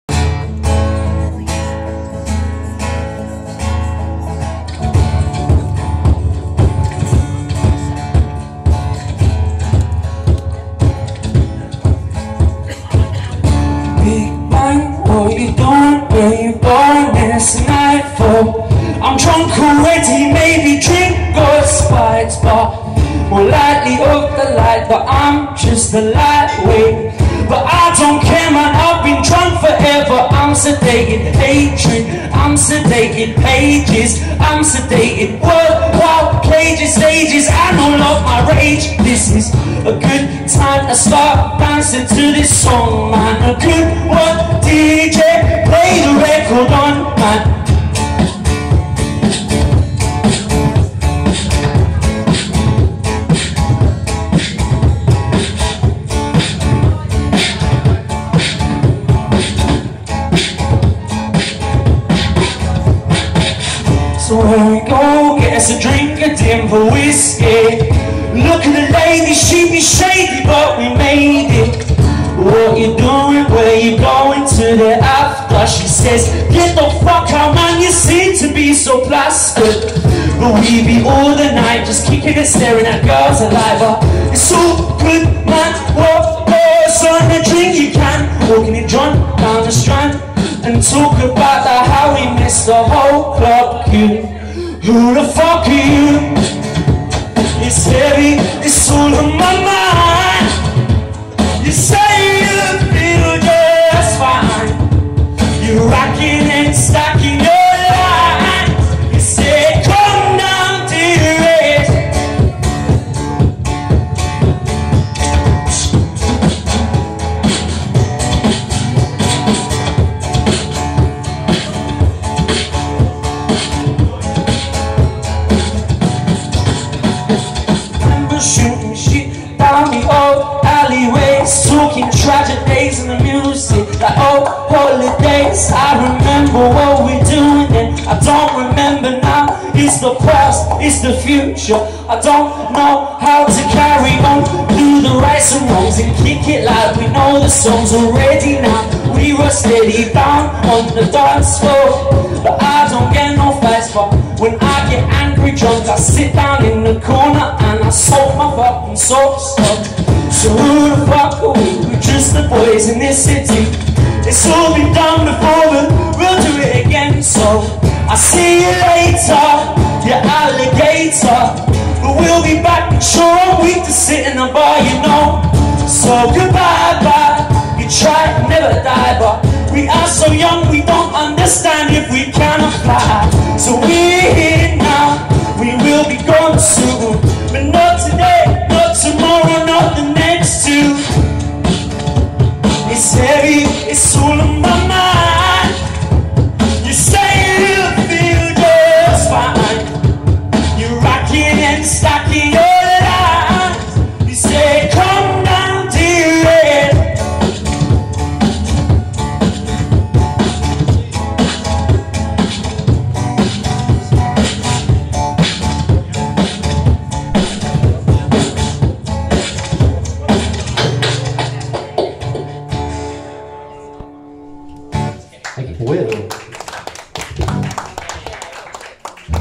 Live Music & Jams At The White Horse Pub In Bedford :)
Live Music & Jams At The White Horse Pub In Bedford :) Monday 08th Of February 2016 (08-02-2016) :)